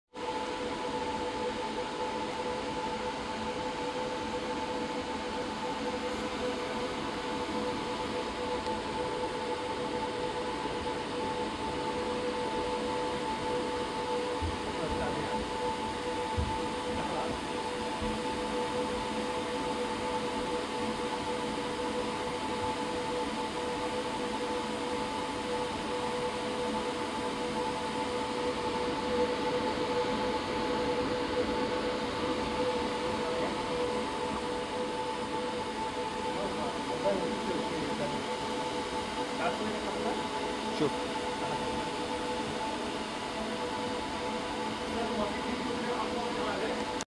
The Sadeem Screen 1.0 / (Test No 2) / By the Omani Inventors Team.
Our second successful test for the SS 1.0
The SS 1.0 is a High-Tech Midair Display System.
The video shows a test that was designed to experiment with our new method of reducing the water dripping effect. thankfully, we where able to reduce significantly the amount of water dripping. The SS 1.0 still need to go into some fine-tuning, in order to get to the optimum performance.
All the thanks to Allah the Almighty, the second test was a complete success, and we have achieved the requited targets.
Now, we have the "Go" to start working on manufacturing the SS 2 to be demoed at COMEX 2013.

Note:
"The SS 1.0 is fully developed and manufactured by The Omani Inventors team, which is a team of inventors sponsored and hosted by ITA"